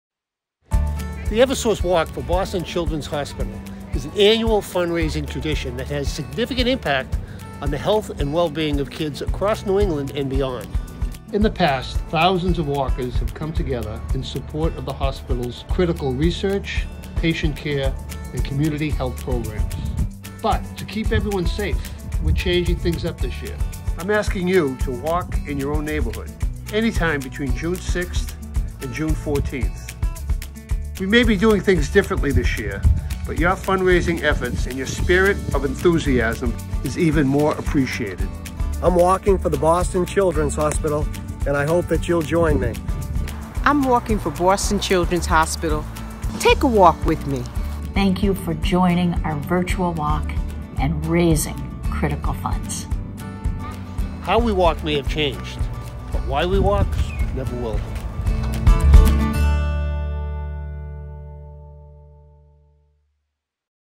2020 Eversource Walk for Boston Children's Hospital
Join us for the 2020 Eversource Walk for Boston Children's Hospital! You can complete your walk anytime between June 6 - June 14 and all proceed go to support the kids at BCH. Share your photos/video on social media using the hashtag WalkforBCH.